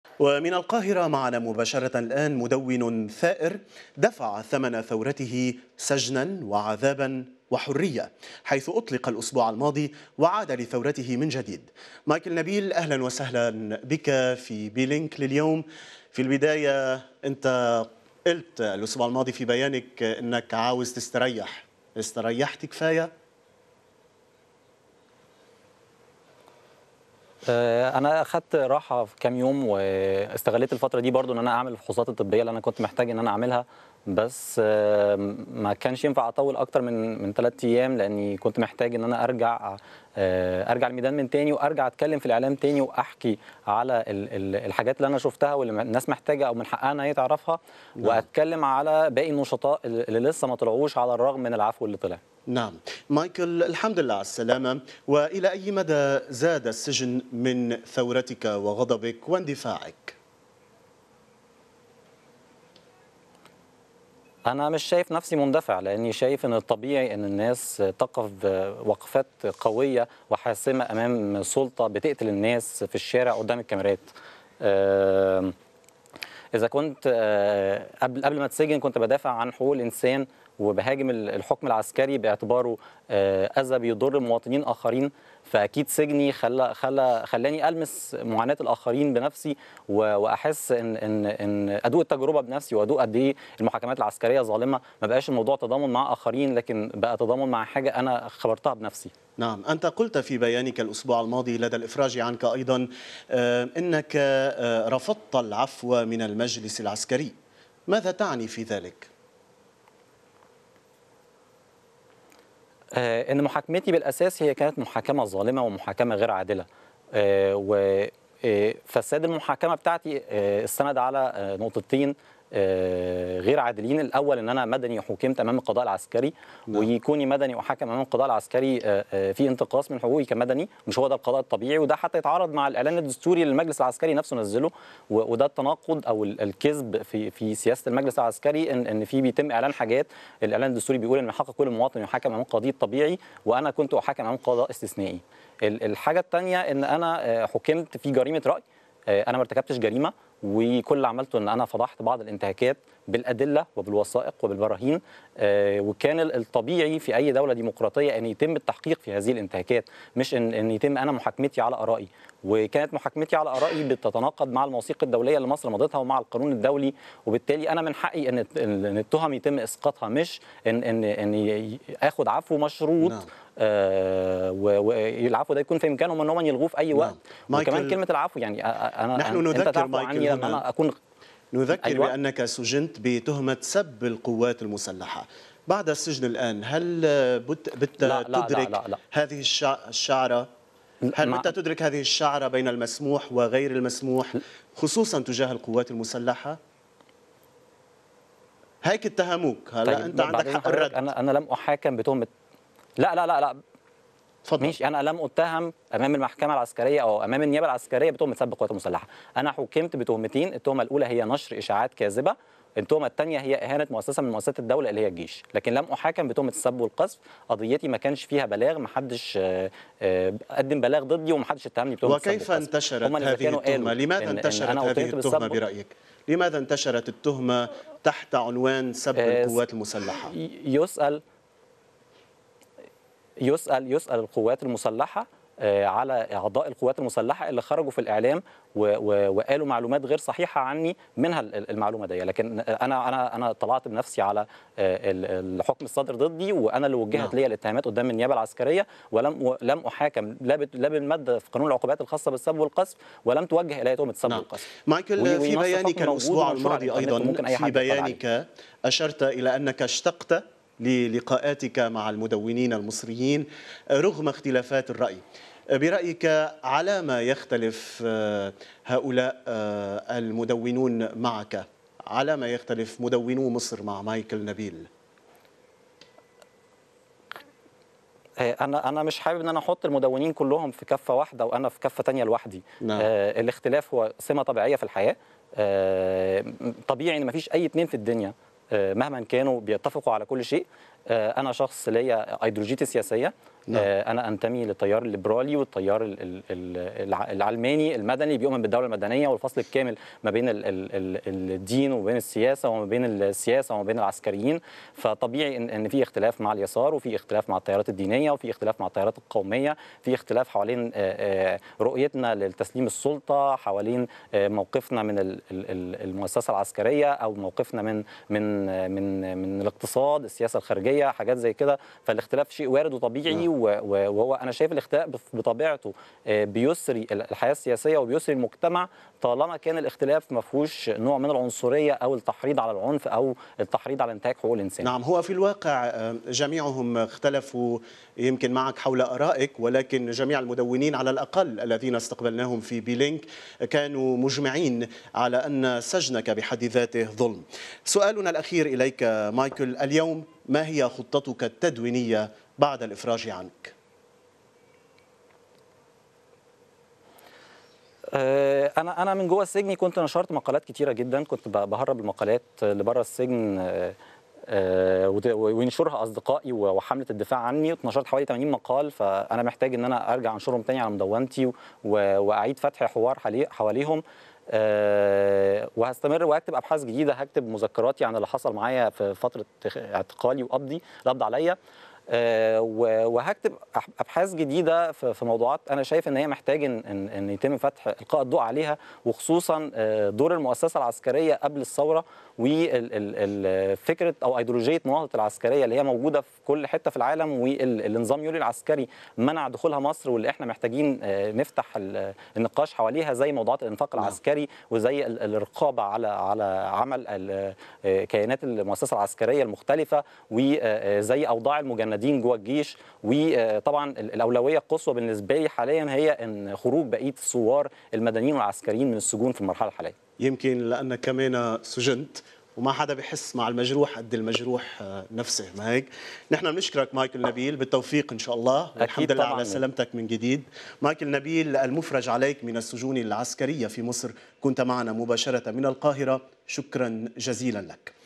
لقاء B.Link مع الناشط السياسي مايكل نبيل
لقاء خاص مع الناشط السياسي مايكل نبيل والذي أفرج عنه بعد أن أصدر المشير طنطاوي القائد العام بالقوات المسلحة قرارا بالإفراج عن 1959 سجينًا بمناسبة عيدى ثورة يناير وعيد الشرطة.